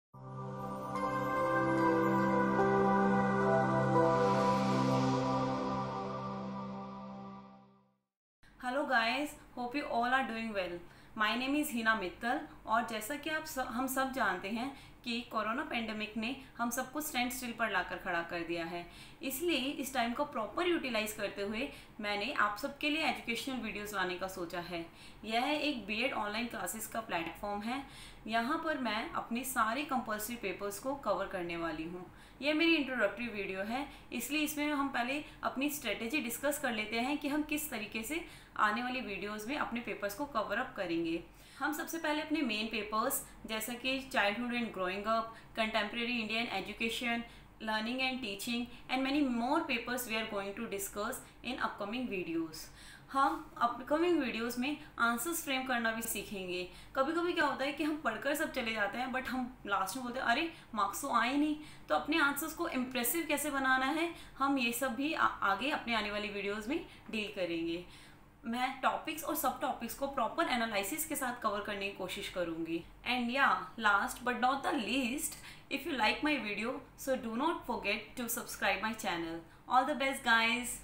B.ED. || Bachelor of education || Introductory video by Heena
This is a Graduation level course for 2 years. It is designed specially for aspiring teachers.
This is my introductory video and I am going to discuss my strategy to cover the entire B.ed syllabus. I will cover all the compulsory papers. There will be proper analysis of all the topics and sub topics covered. I will give specific questions related to the topics as well.